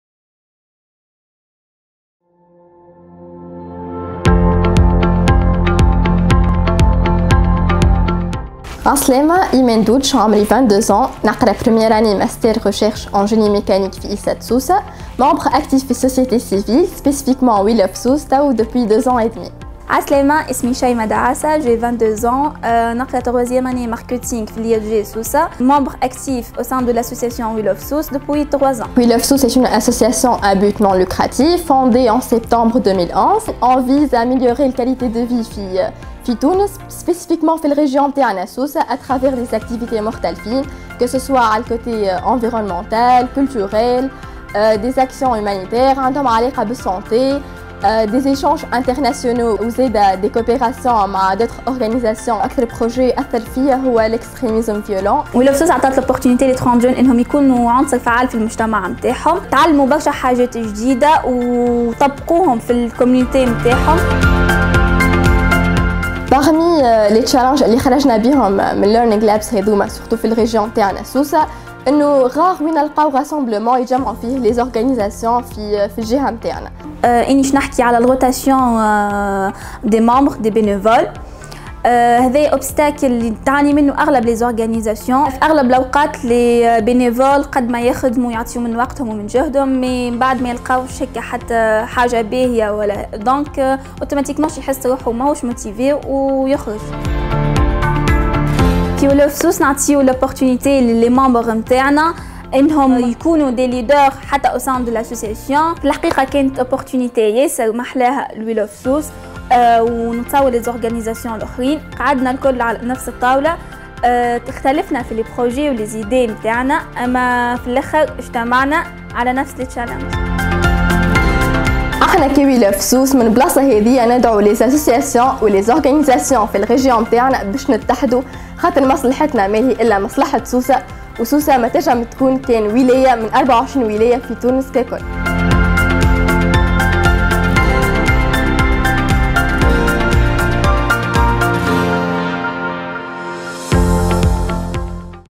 CSO Challenges in Sousse, Tunisia
النشاط الثاني من برنامج النشطاء_الجدد إلي تقودو شبكة الابتكار للتغيير- الشرق الأوسط وشمال إفريقيا Innovation for Change - Middle East and North Africa و تنفيذ السبايس والمرة هذي معانا إيمان_دودش و شيماء_دعاسة من سوسة اعظاء فاعلين و ناشطين في جمعية WE LOVE SOUSSE يشاركونا تجربتهم معانا في مختبرات التعلم ورواية القصص المجتمعية "Community Storytelling and Learning Labs to change CSOs narratives" ويحكولنا أكثر على جمعيتهم ونشاطها
Imen Doudech Imen_Doudech & Chaima_Daassa Shaiima Da two active members of WE LOVE SOUSSE organization who participated together with us in the 2nd activity of The_New_Activists Program "Community Storytelling and Learning Labs to change CSOs narratives" led by Innovation for Change - Middle East and North Africa and implemented by ELSpace.
let’s put attention to what they want to share with us about their organization and their participation.